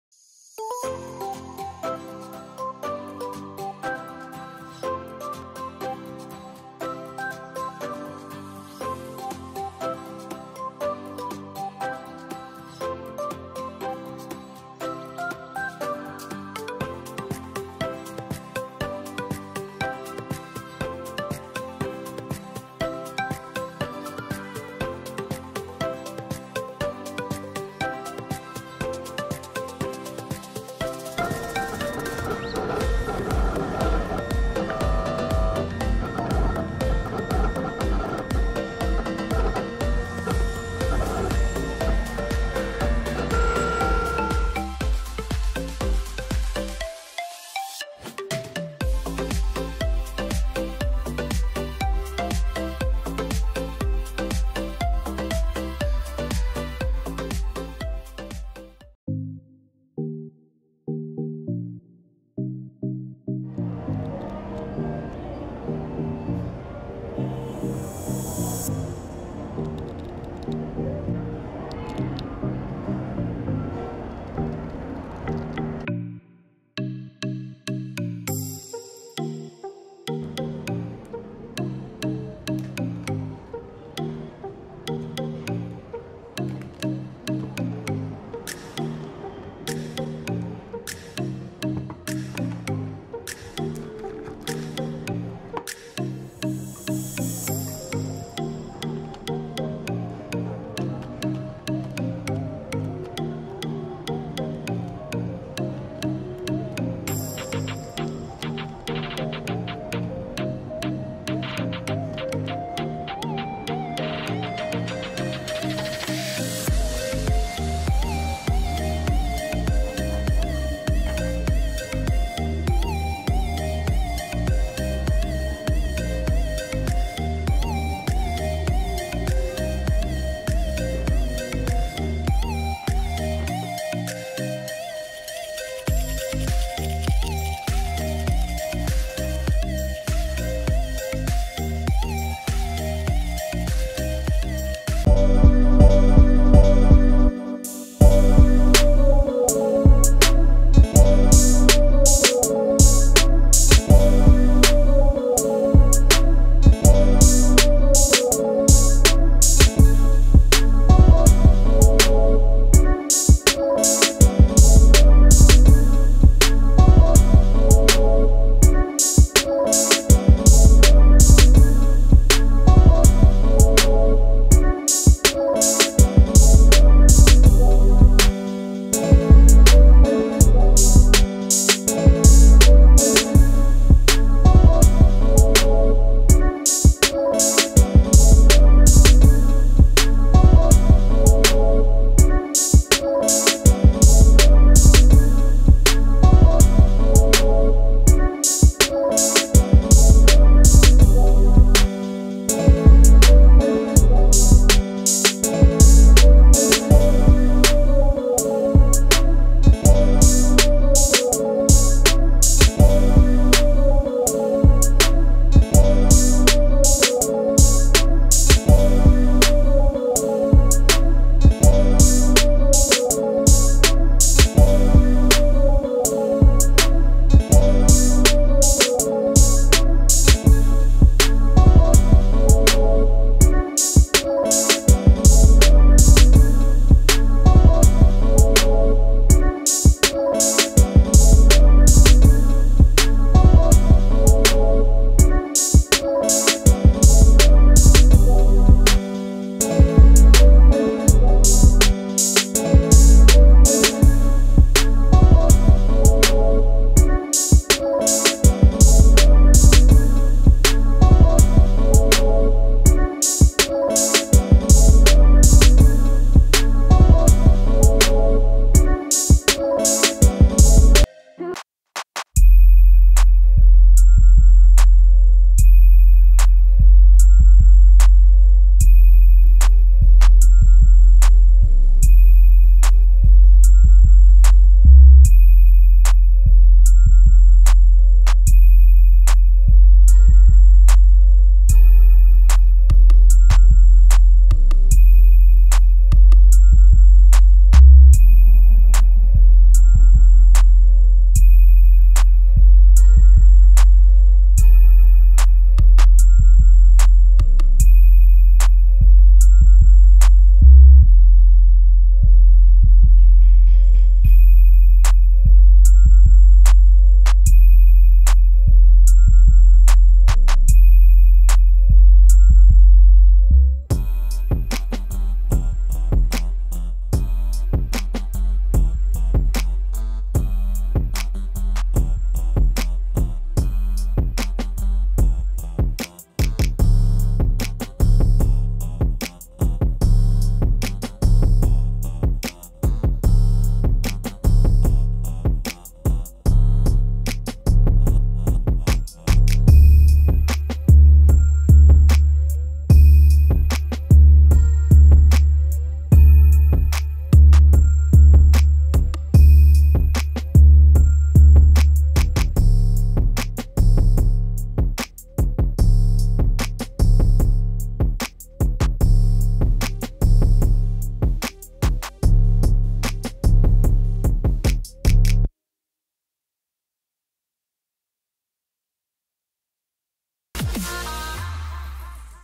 How To Make Circuit Board Using Vinyl Sticker
Im using vinyl sticker and Cutter Plotter to make Circuit board

Thanks